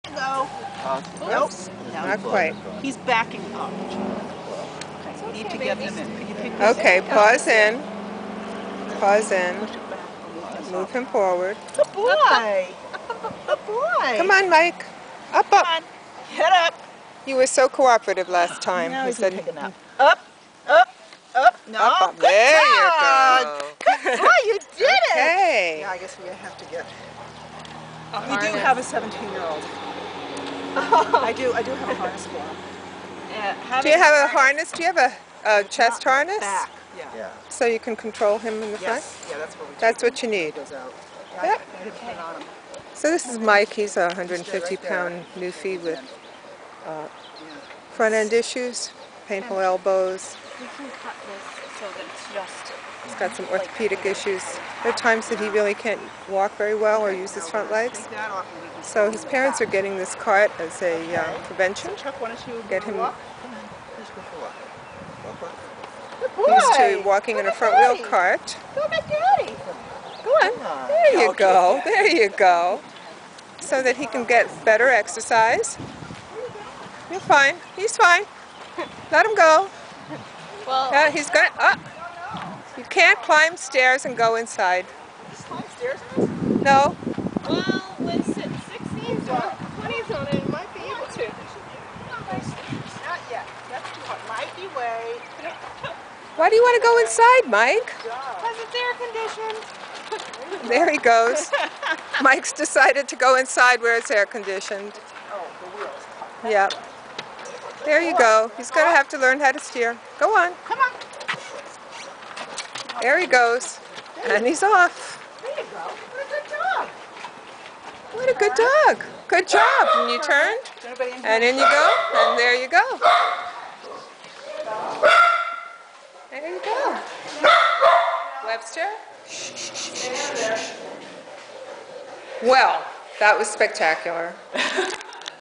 A front wheel Eddie's Wheels dog wheelchair for Mykroft
Mykroft has severe arthritic problems with his front legs that makes it hard for him to go for long walks.  We weren't sure he'd take to a front wheel cart, but at least at his fitting at Eddie's Wheels, he took right off in his wheels.